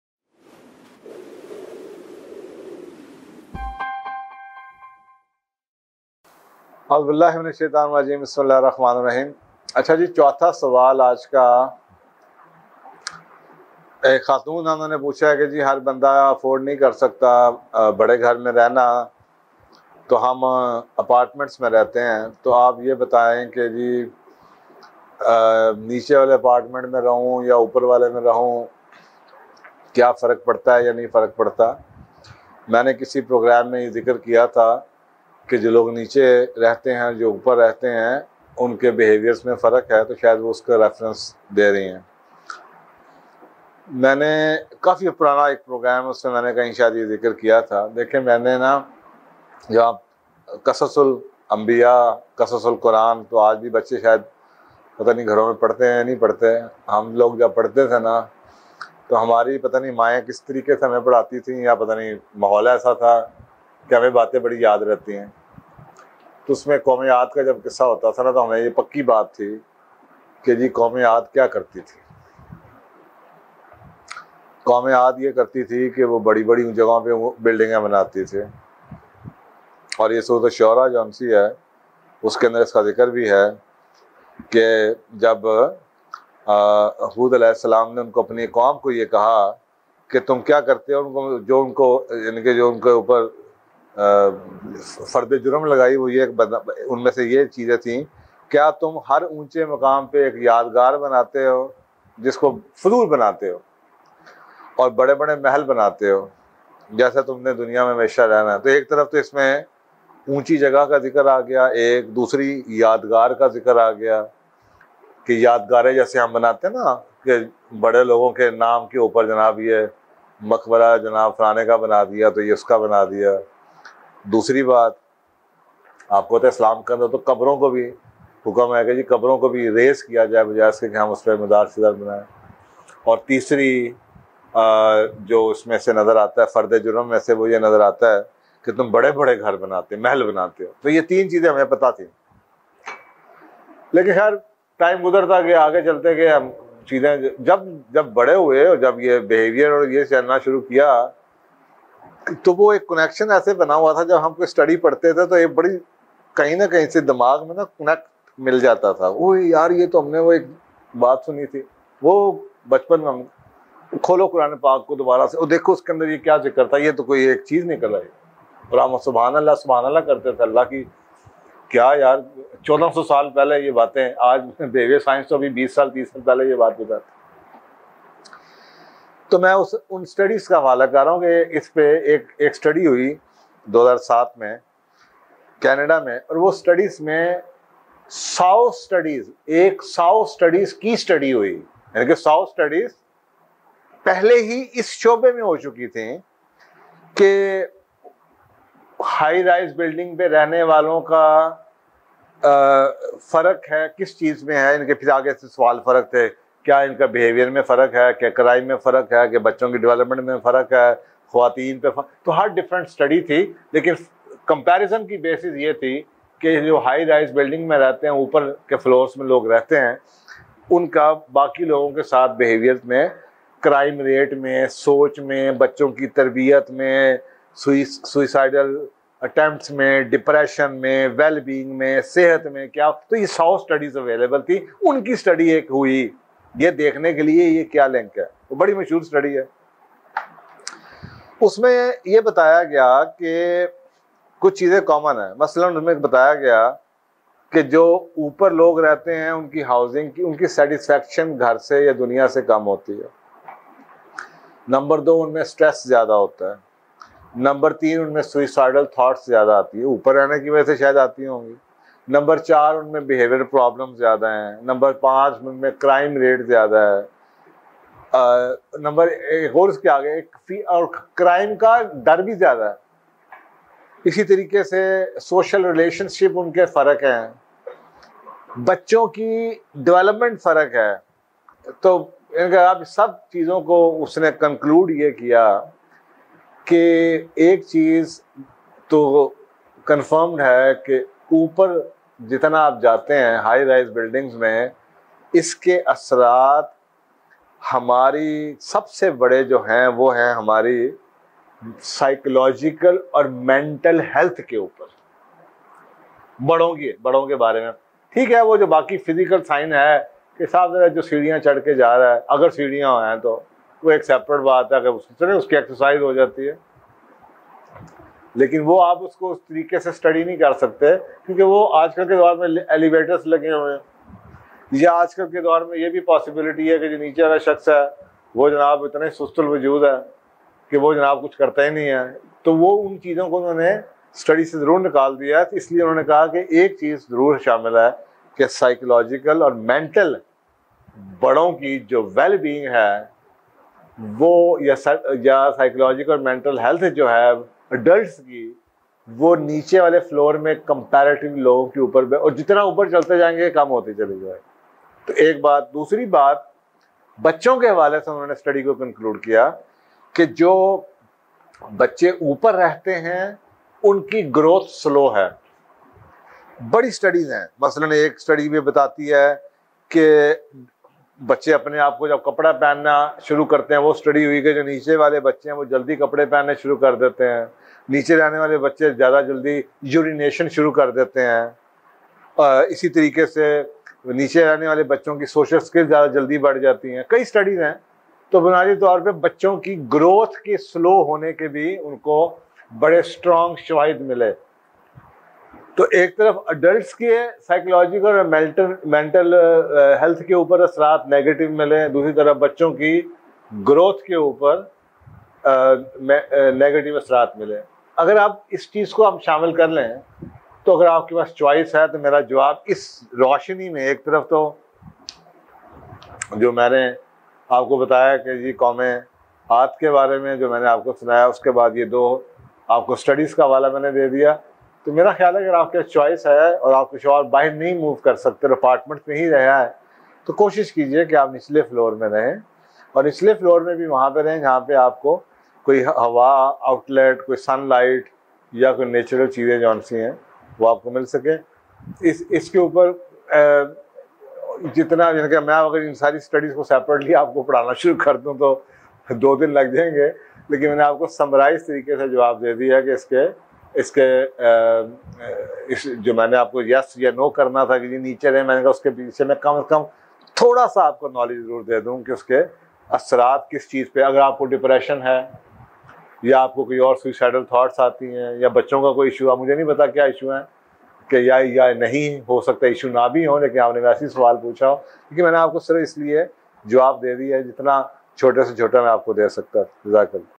Side effects of living in apartments and high rise buildings | STRESS | ANXIETY | FAIEZ SEYAL
There are numerous research studies available on living in apartments and high rise buildings,
In the video, Sir Faiez Seyal explained beautifully about the side effects of health which includes, stress, anxiety, and much more by living in apartments and high rise buildings.

For Appointment or Details of Faiez Hassan Seyal's Programs call at +92-333-4VERITA (i.e. 837482)